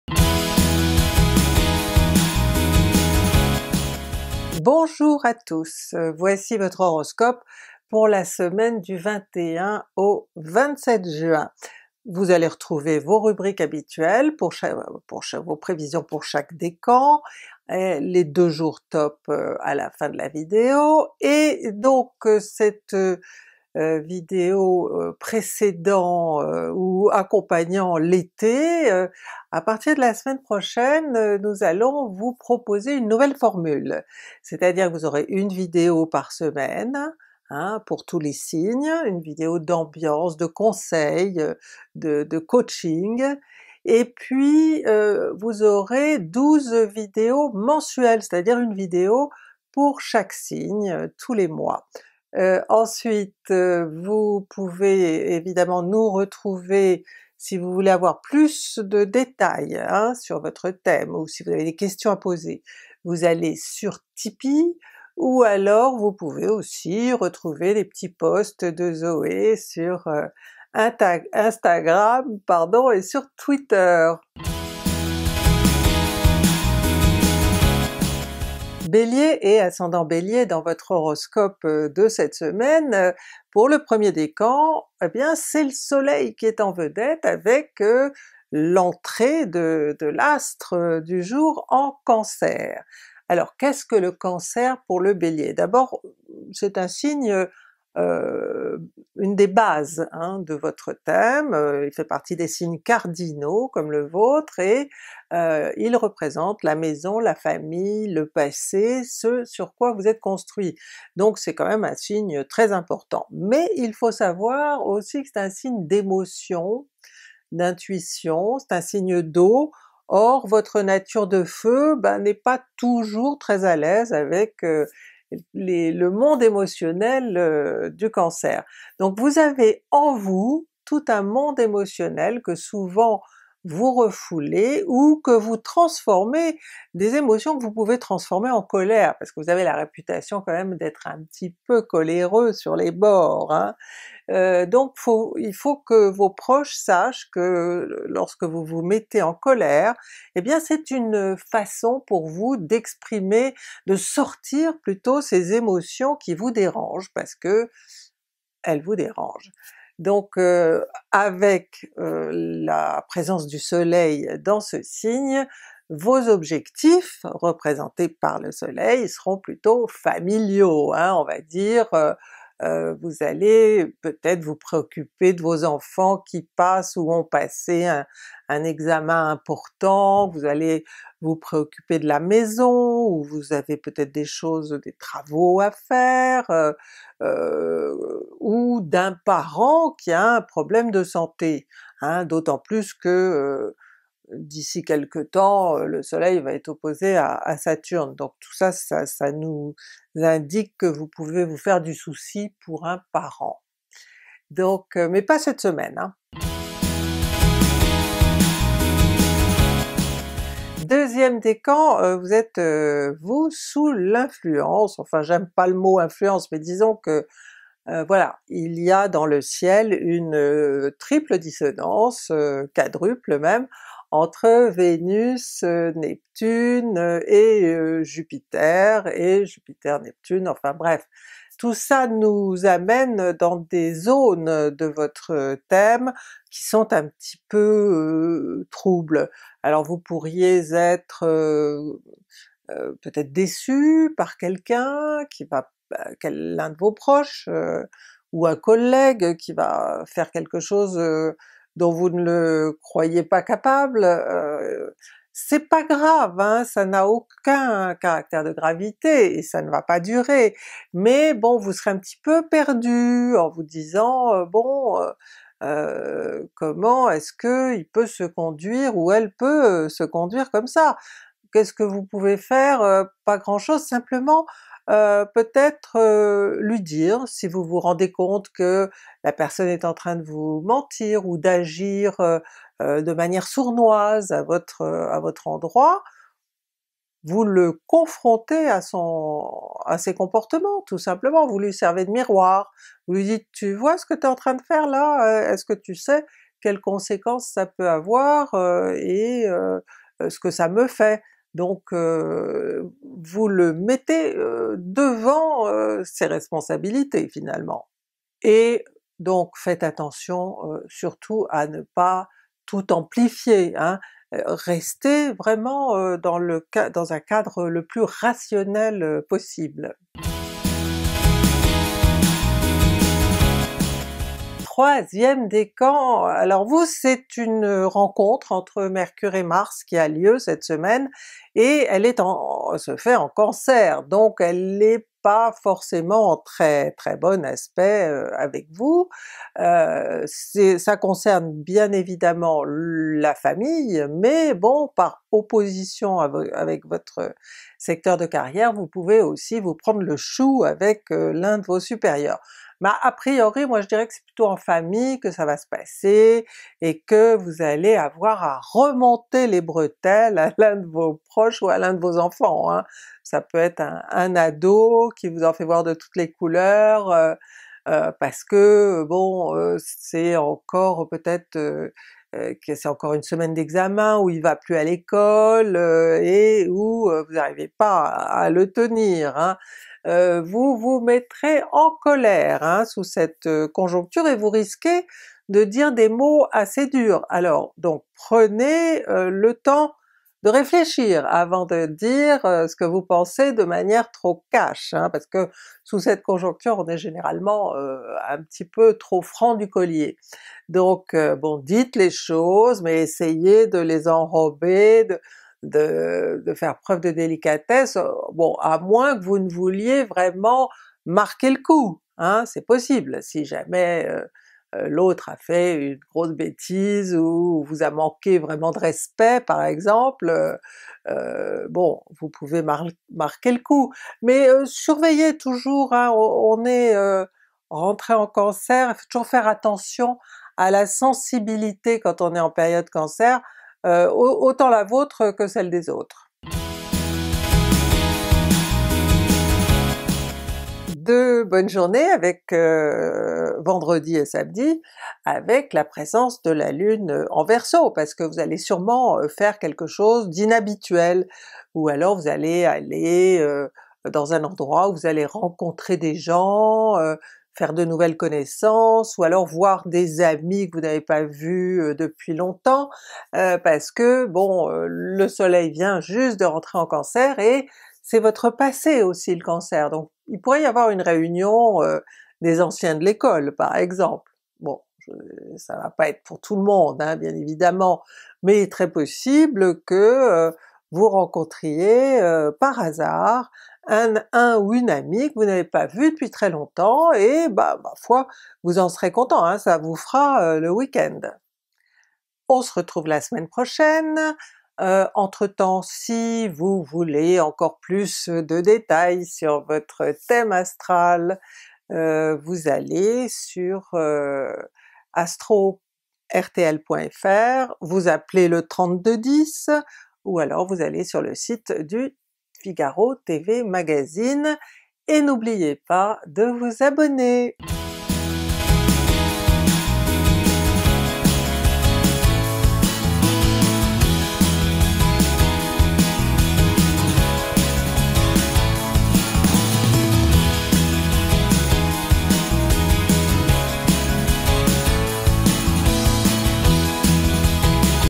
Horoscope Bélier ♈️ du 21 au 27 juin 2019 ✨
BÉLIER ♈️, quel est votre HOROSCOPE pour la SEMAINE du 21 au 27 juin 2019? Allez-vous passer une bonne semaine? Quel sera votre JOUR TOP? Comment l'ASTROLOGIE peut vous guider?

Je suis Christine HAAS, ASTRO-PSYCHOLOGUE (pour RTL et le TV Magazine). Découvrez votre HOROSCOPE pour la semaine du 21 au 27 juin 2019 si vous êtes BÉLIER ♈️ ou ASCENDANT BÉLIER ♈️.
Je vous conseille de regarder l'HOROSCOPE de votre signe et celui de votre ASCENDANT pour plus de précision. Et je vous rappelle que ces prévisions sont générales car je ne dispose pas de votre thème de naissance. Elles sont donc à prendre avec modération.